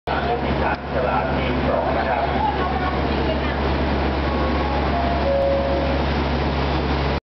Passanger train in Phitsanulok, Thailand
A "super express" train assembled from Daewoo-made cars on a meter gauge track.